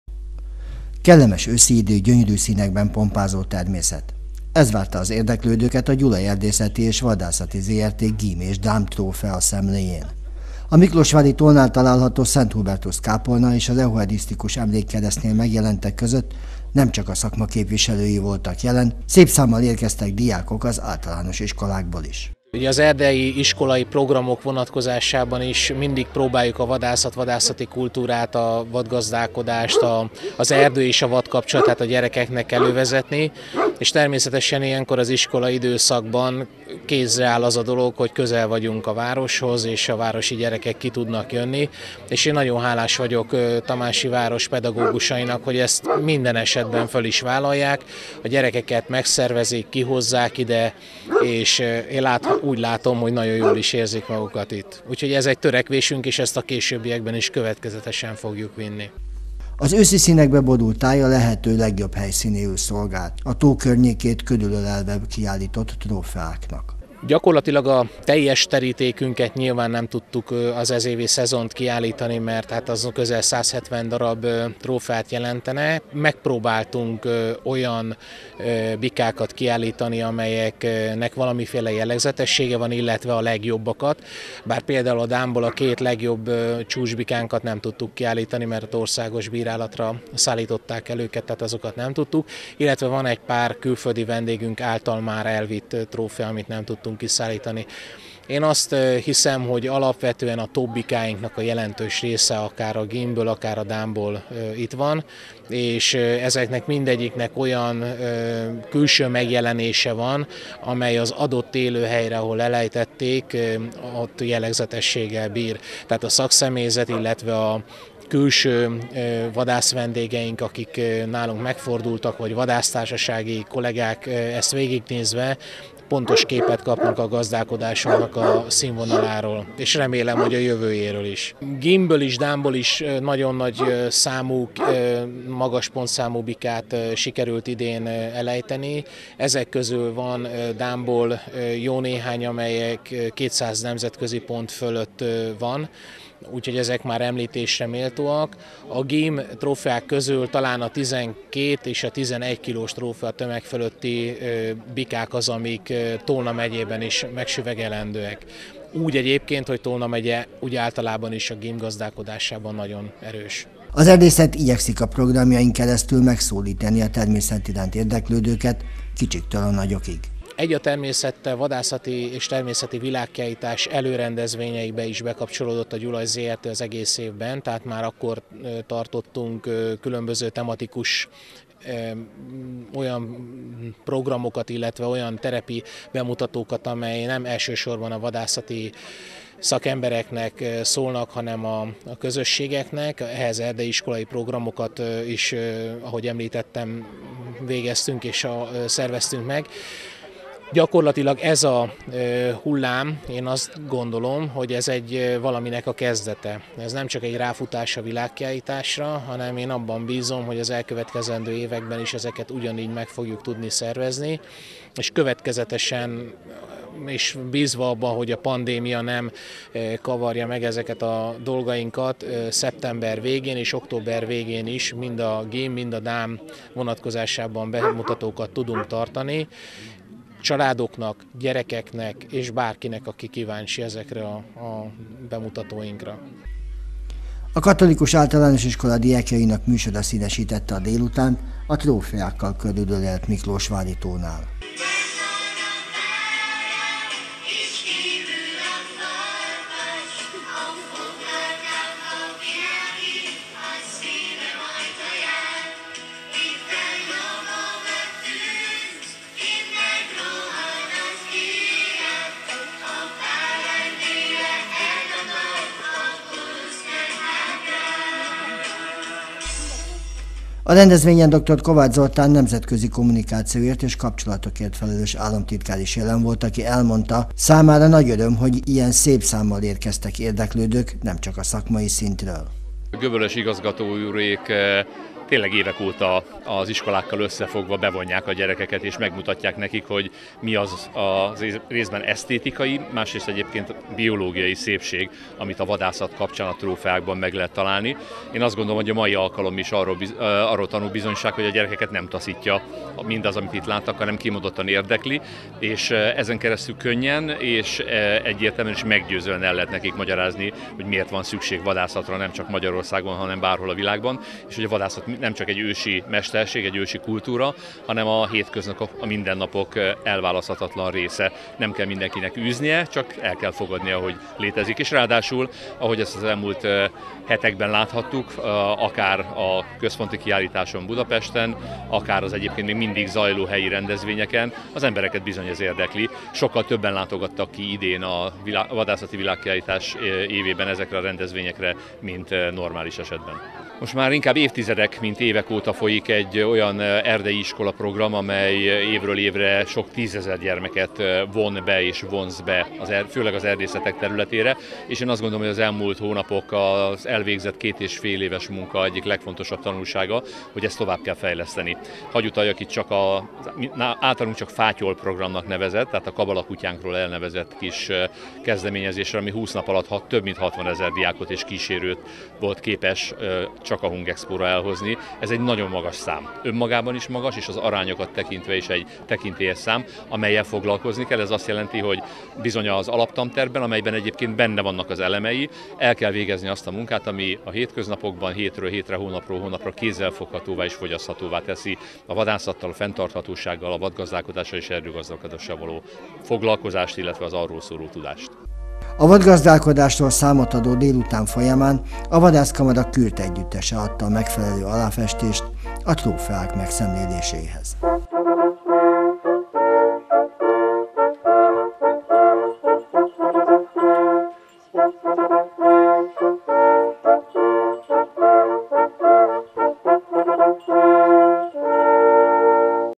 Dám- és gímtrófea szemle Tamásiban
A vadásztrófea fogalmán nem csupán agancsot, agyarat és szarvat értünk. Ennél sokkal nagyobb jelentéssel bír egy vadász számára., Számos emlék társul hozzá. Noha az emlékek gyarapítását  nem, de a lehetőséget a legszebb trófeák megszemlélésére biztosította a Gyulaj Erdészeti és Vadászati Zrt. A vadászok életében igazi ünnepnapnak számító eseményre invitálták az érdeklődőket.